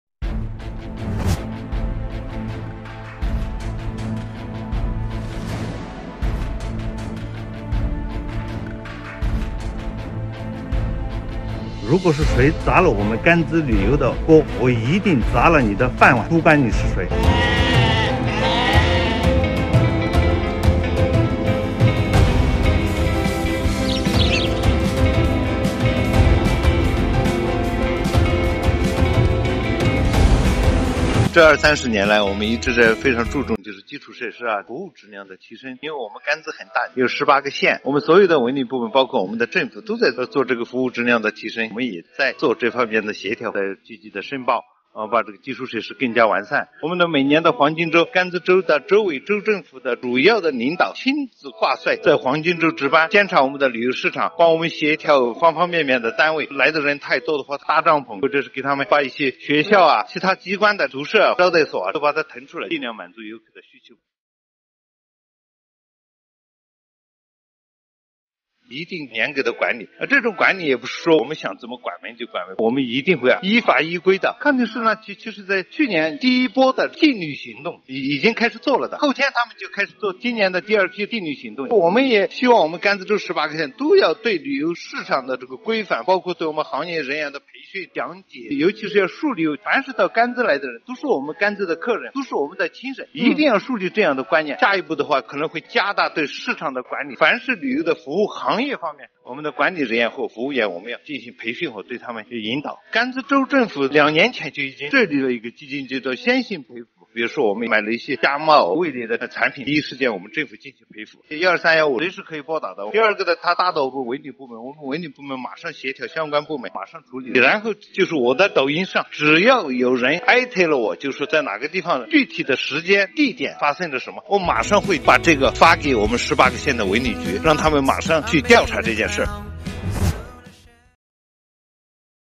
劉洪回應處理問題酒店是否依法質疑：多部門調查，依法依規進行
2月26日，四川甘孜。網友投訴康定一酒店引關注，甘孜文旅局局長劉洪稱“誰敢砸甘孜旅游的鍋，我就砸你的飯碗”。27日，針對“處理酒店事件”少數網友質疑，劉洪稱，處理是依法依規進行的，為保障旅游市場正常運行。February 26, Ganzi, Sichuan. Netizens complained that Kangdingyi Hotel attracted attention. Liu Hong, director of Ganzi Culture and Tourism Bureau, said that "whoever dares to break the pot of Ganzi tourism, I will break your job". On the 27th, in response to the query of a few netizens about "handling the hotel incident", Liu Hong said that the handling was carried out in accordance with laws and regulations to ensure the normal operation of the tourism market.